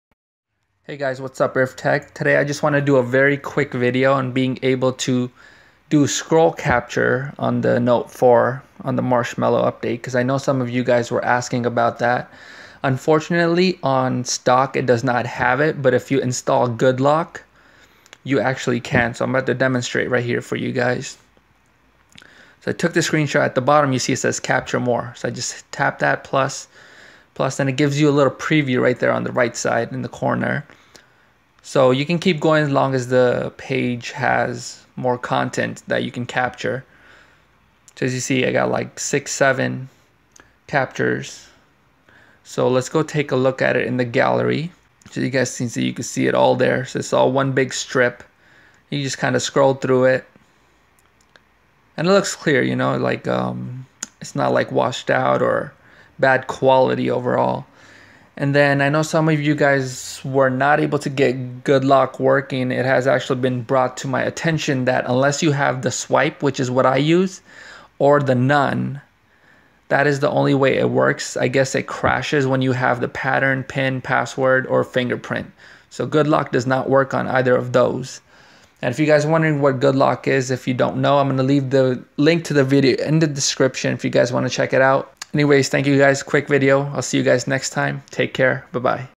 Galaxy note 4 Scroll Capture and Good Lock Tip!
Scroll capture works with the screen write with the s pen as well. My Mistake :D